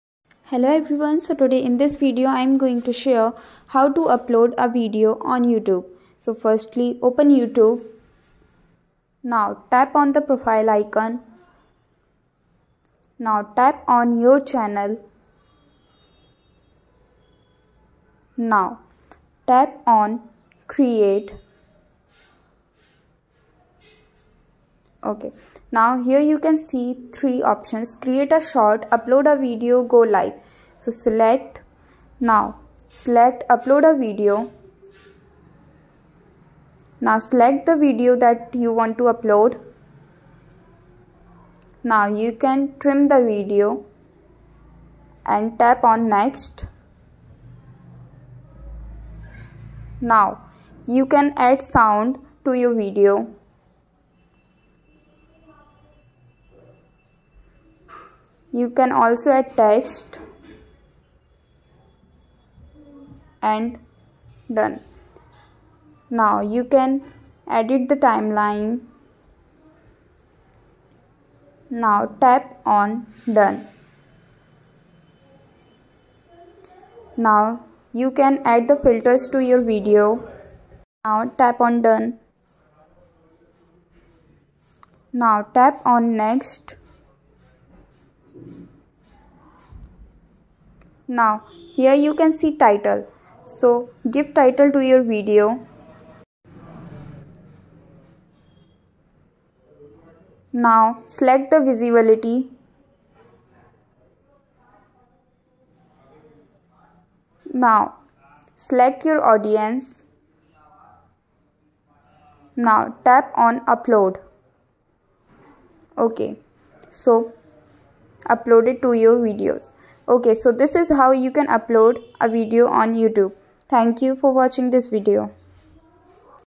How to Upload Videos to YouTube with an Android Phone
In this video I am going to show How to Install YouTube App on iPhone, iPad, or iPod Touch. So let us see How to Use the YouTube App on an iPhone.